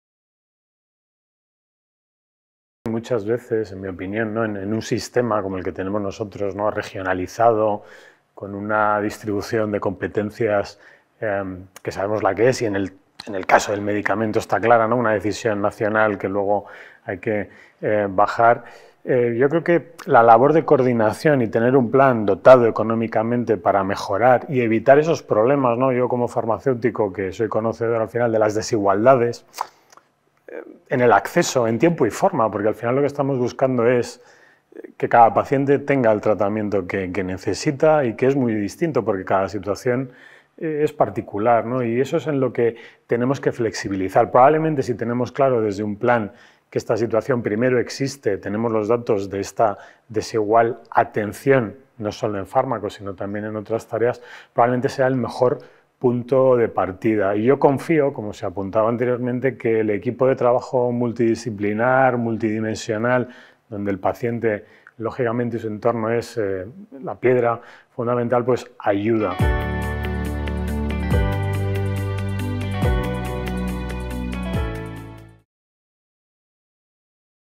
José Manuel Martínez-Sesmero, director de Innovación de Sociedad Española de Farmacia Hospitalaria
El Plan General de Migraña, prometido por el Ministerio de Sanidad constituye el "mejor punto de partida" a ojos de José Manuel Martínez-Sesmero como director de Innovación de la Sociedad Española de Farmacia Hospitalaria, al dar cuenta de las "desigualdades en el acceso en tiempo y forma" que existen en el sistema, "no solo en fármacos sino también en otras tareas".

No obstante, dicha estrategia deberá estar adaptada a las necesidades de "cada comunidad", dada la existencia de regiones "mucho más extensas" pero sin los recursos sanitarios y grandes hospitales que alberga la Comunidad de Madrid, ha suscrito Porta-Etessam.